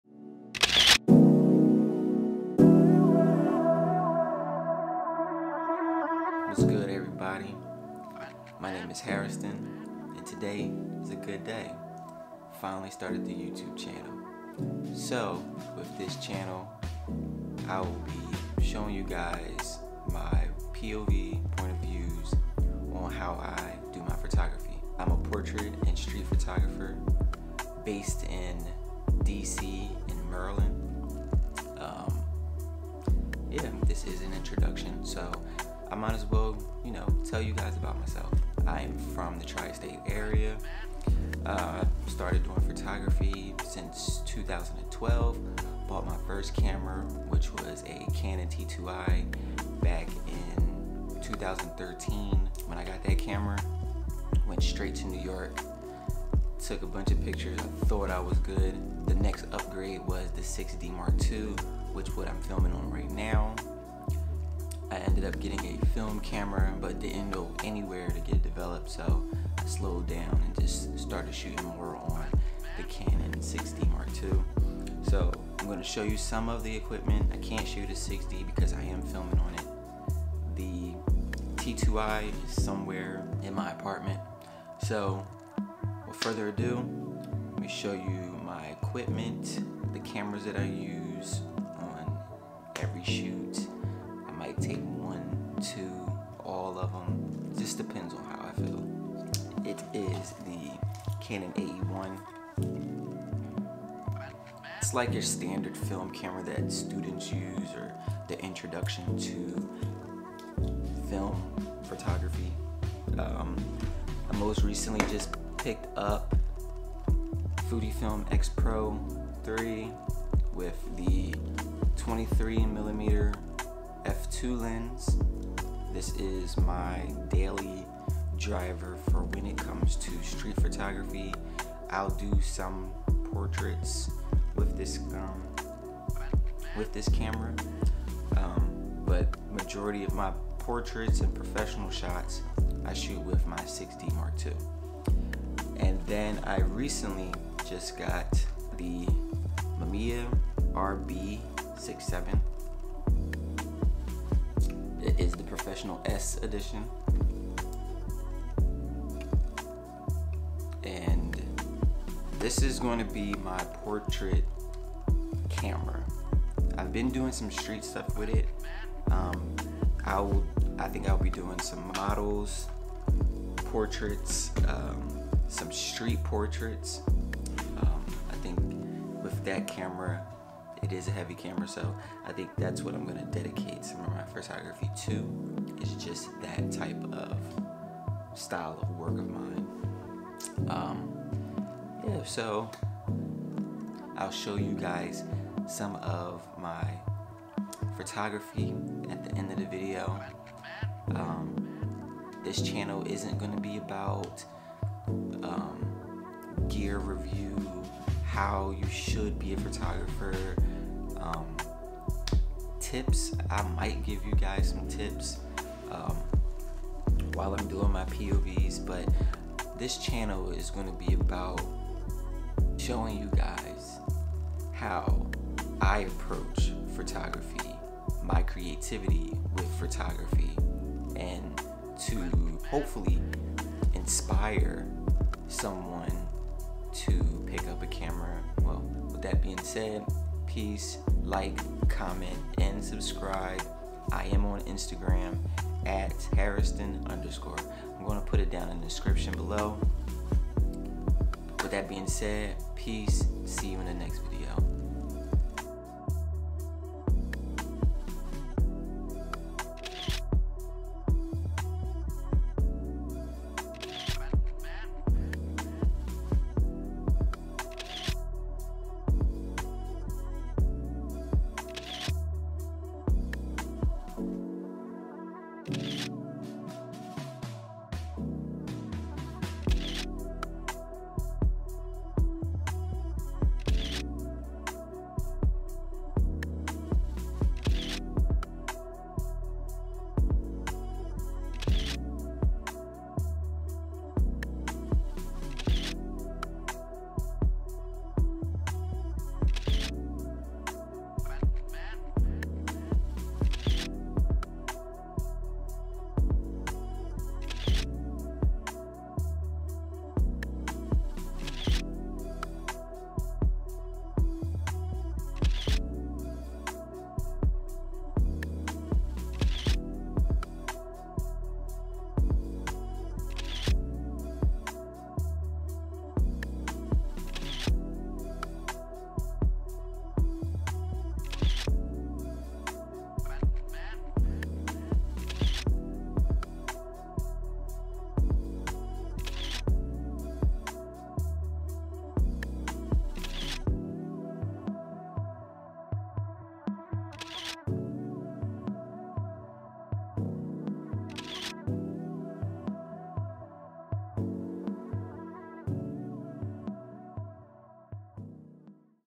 Street Photography: Introduction to YouTube
Welcome to my first video on Youtube. In this video I tell you guys  little about myself, the cameras I use and what this channel will be about.

"Photography is shown at the end go the videos"